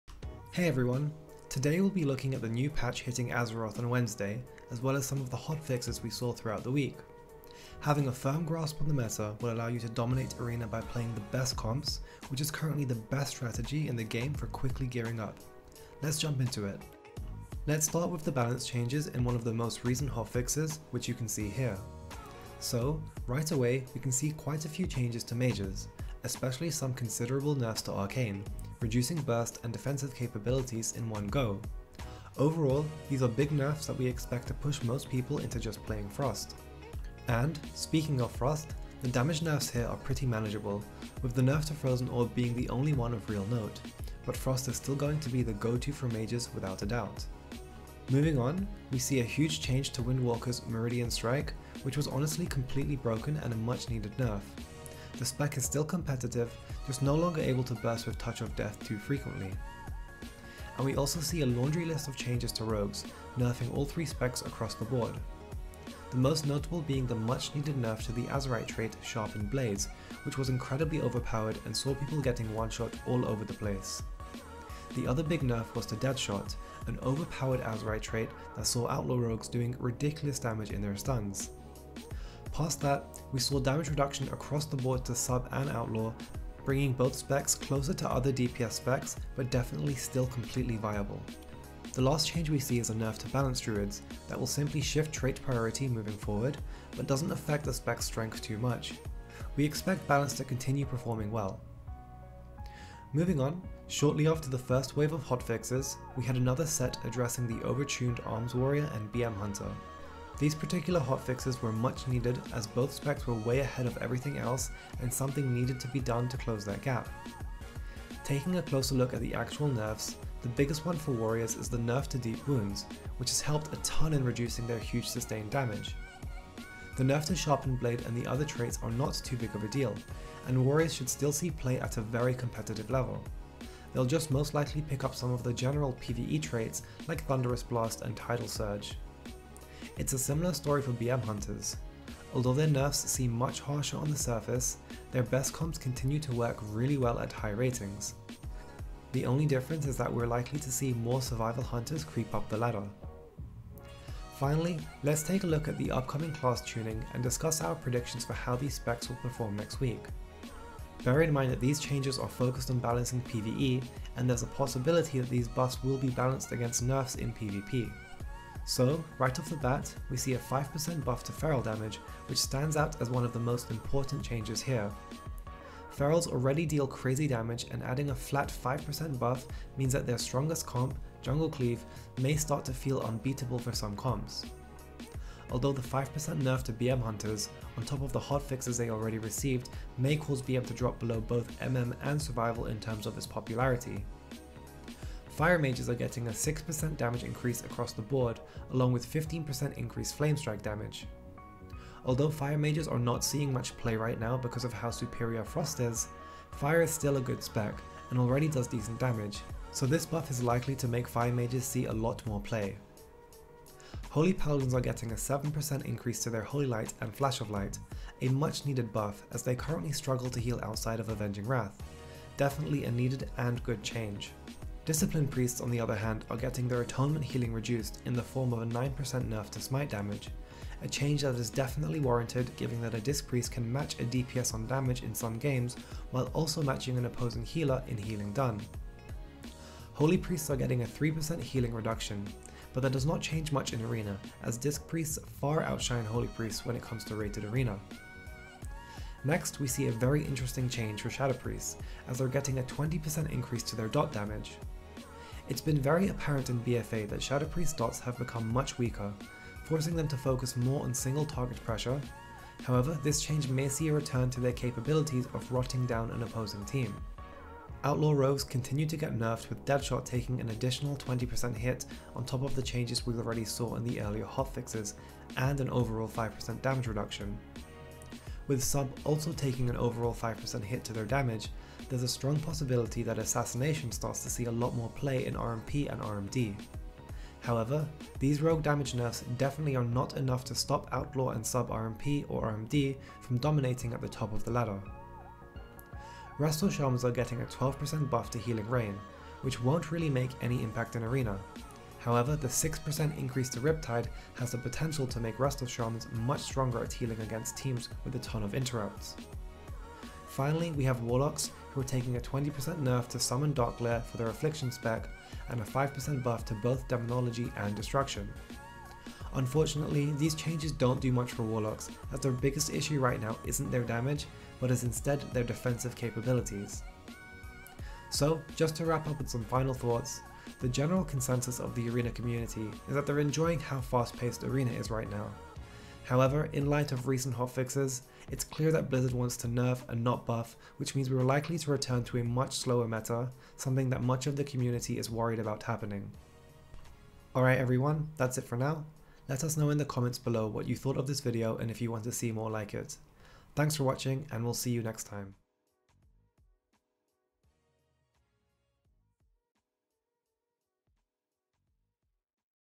BfA 8.0.1 PvP Patch Notes Review & Predictions: Season 1 Week 2
Video Description:
Hey guys! In this video we're going over the most recent hotfixes and giving you our predictions for the upcoming class tuning.